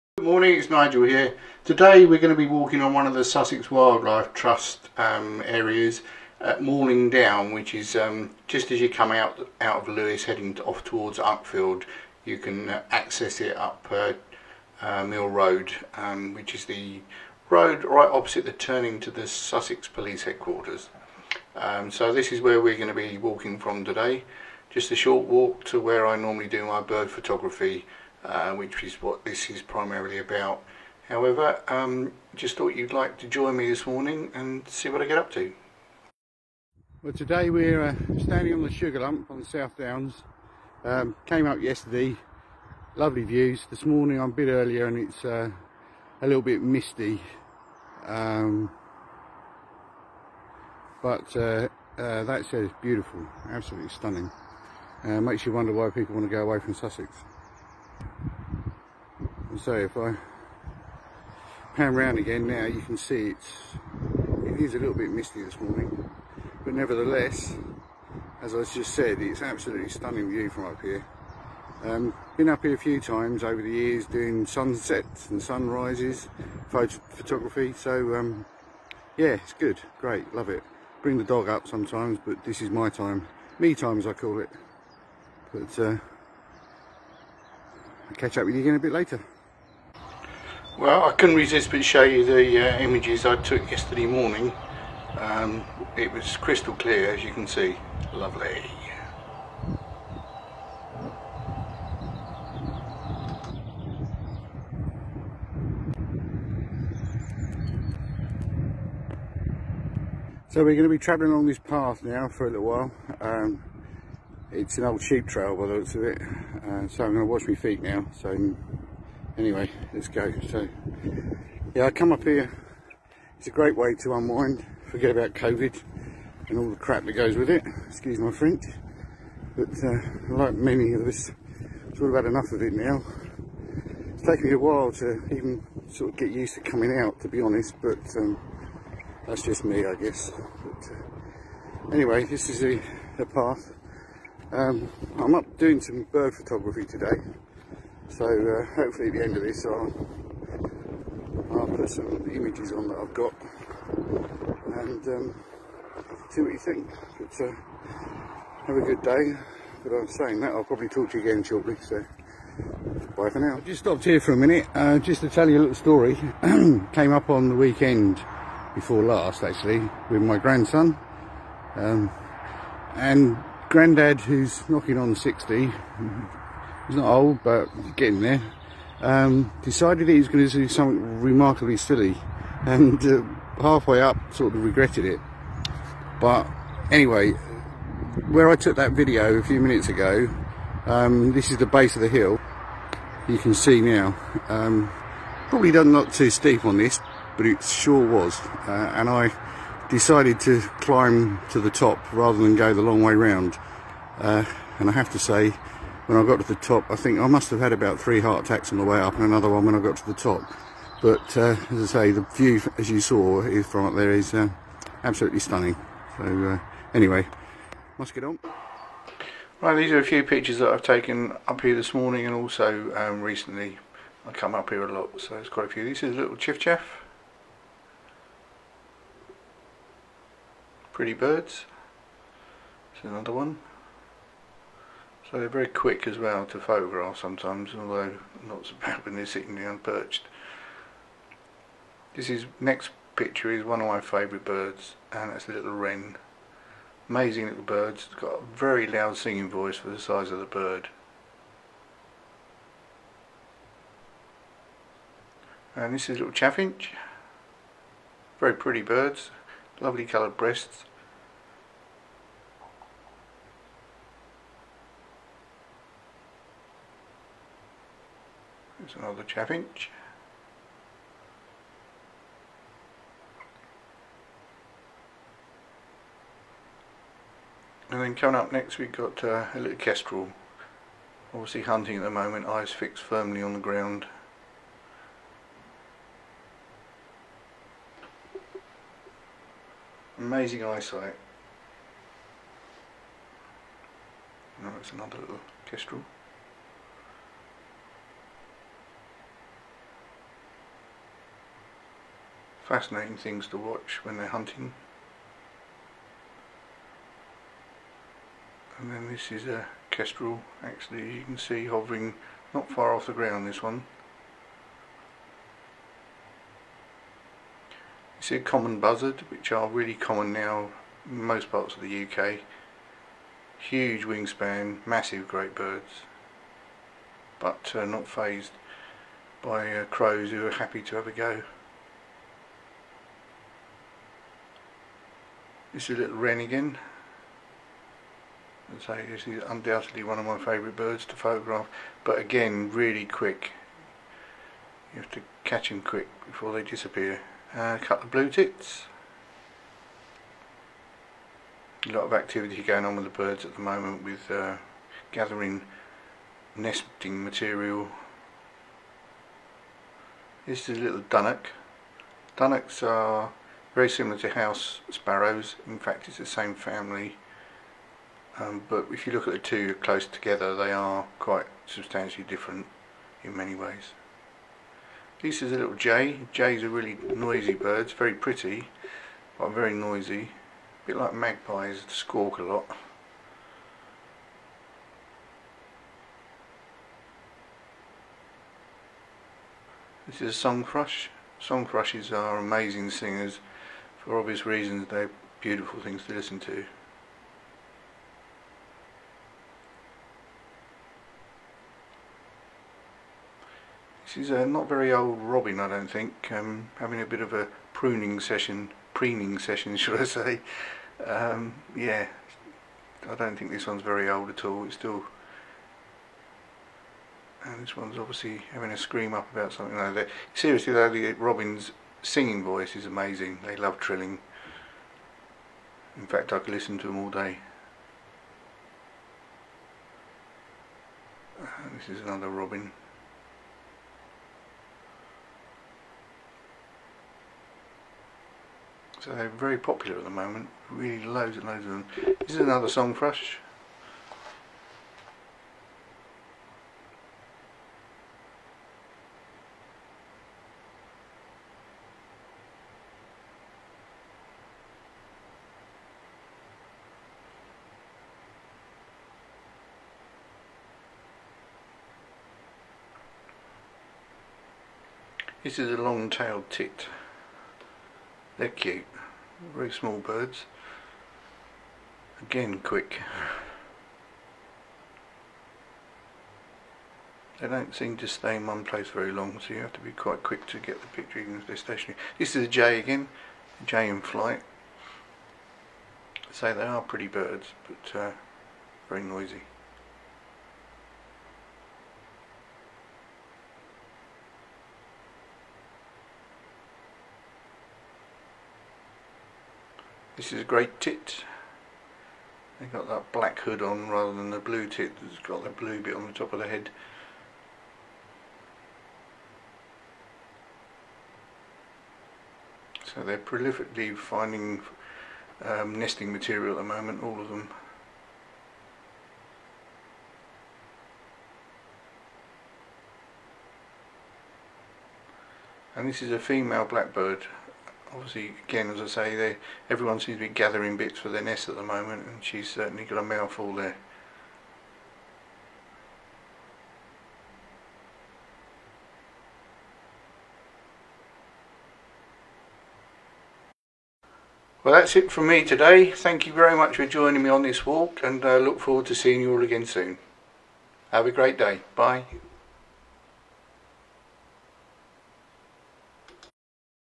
Malling Downs Nature reserve East Sussex
Short walk including a few of my bird photographs and Sunrise image